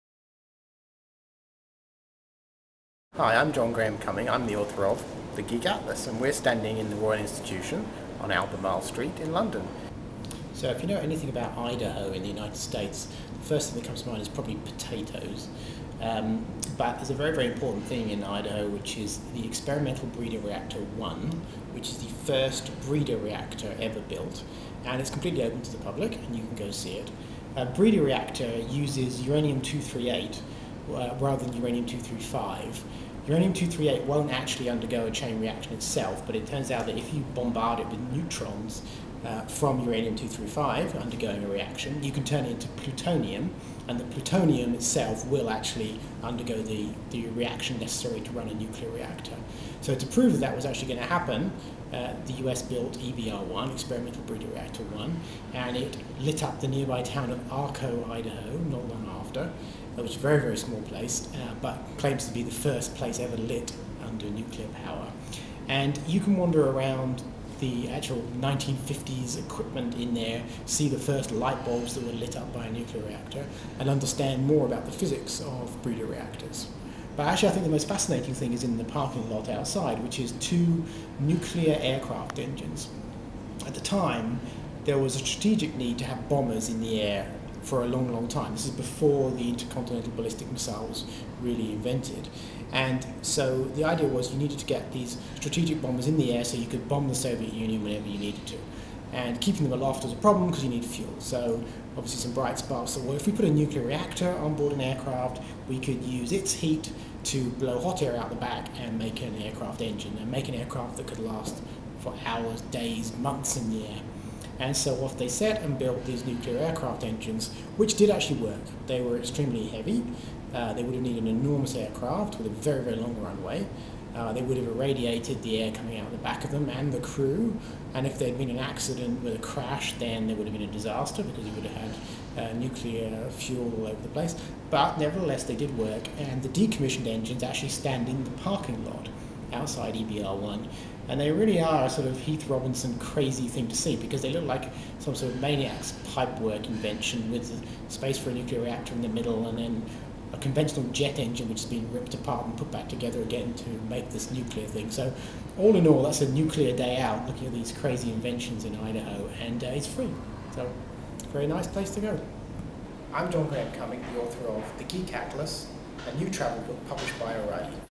Geek Atlas - No1 Breeder Reactor, Idaho
John Graham-Cumming, the author of The Geek Atlas, talked to Craig Smith of O'ReillyGMT about Experimental Breeder Reactor No 1, Arco, Idaho, USA.